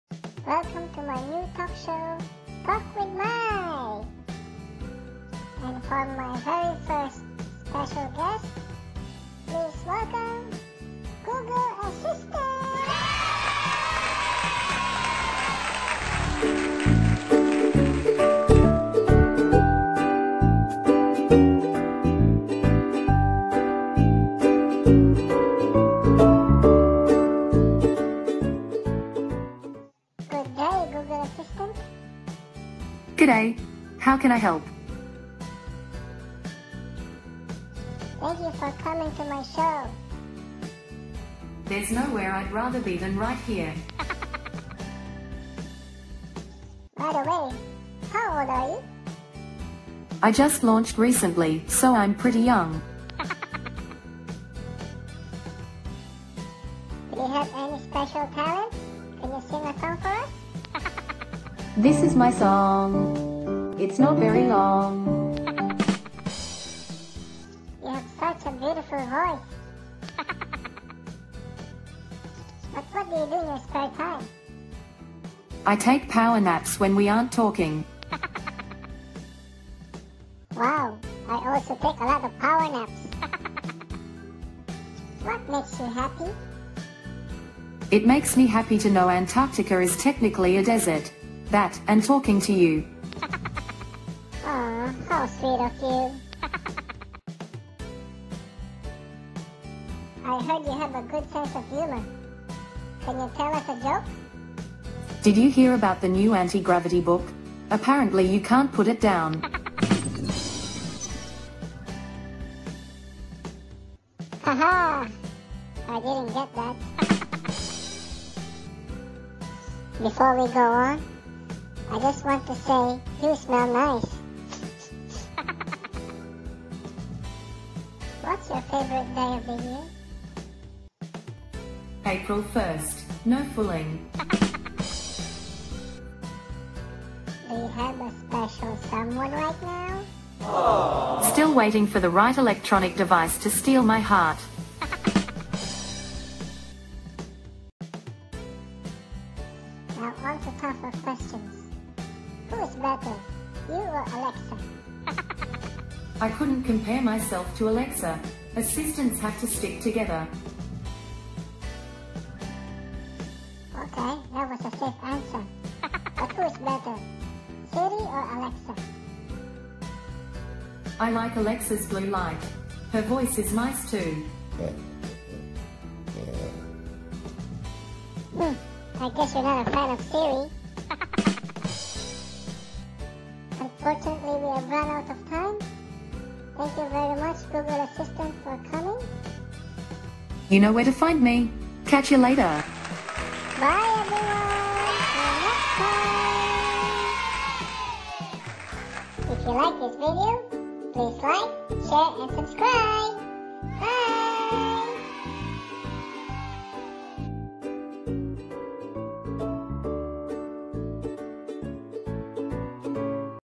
MY DOG REACTS TO GOOGLE ASSISTANT - FUNNY FRENCHIE BULLDOG PUPPY VLOG 008
Mai has new talk show! Her first guest? None other than Google Assistant! Mai will ask the toughest questions in this exclusive interview!!!